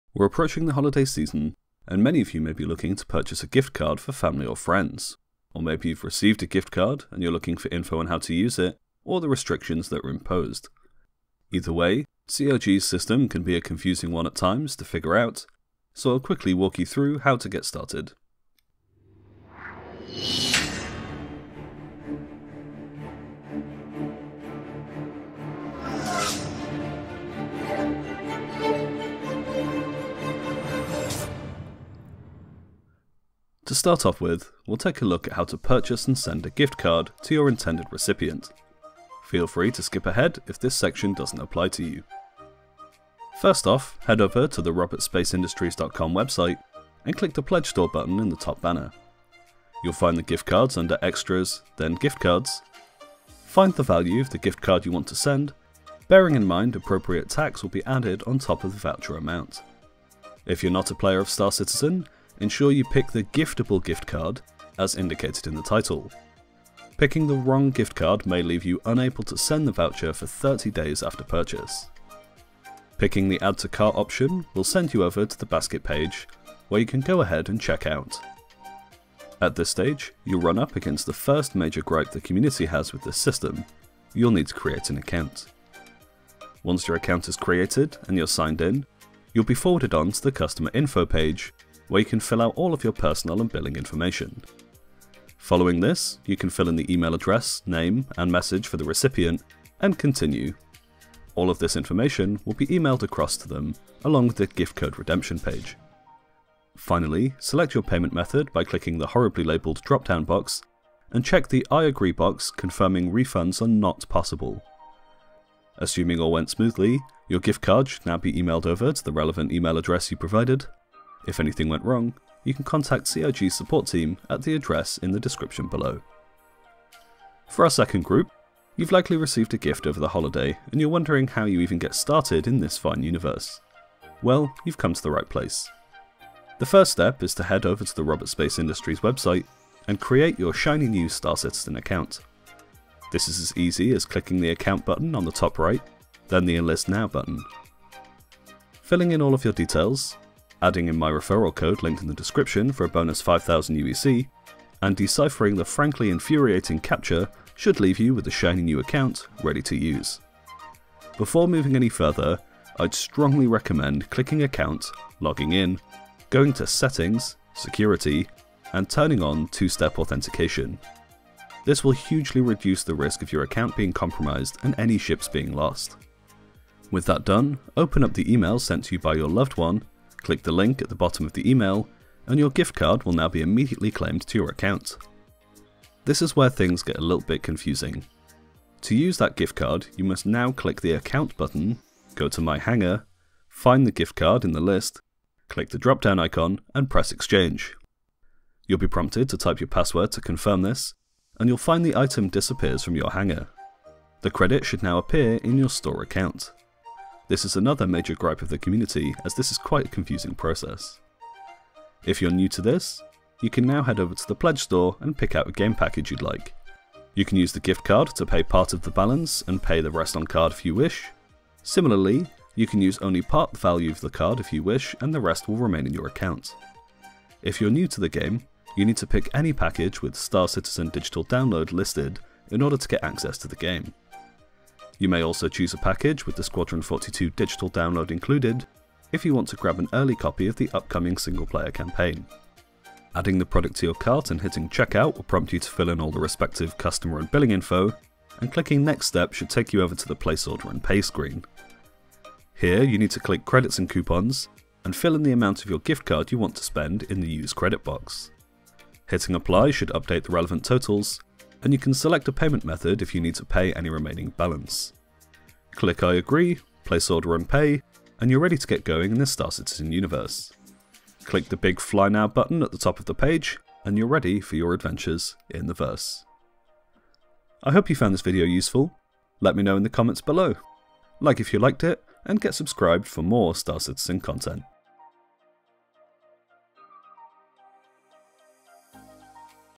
How to buy and use Gift Cards - Star Citizen
With the upcoming holiday season, I've prepared a short video on how to buy gift cards for friends and family, and how they can navigate the rather confusing RSI site to redeem them.

Gifts Cards in Star Citizen can be confusing. Let's take a look at how they're bought and claimed, and how you can make the most this holiday season!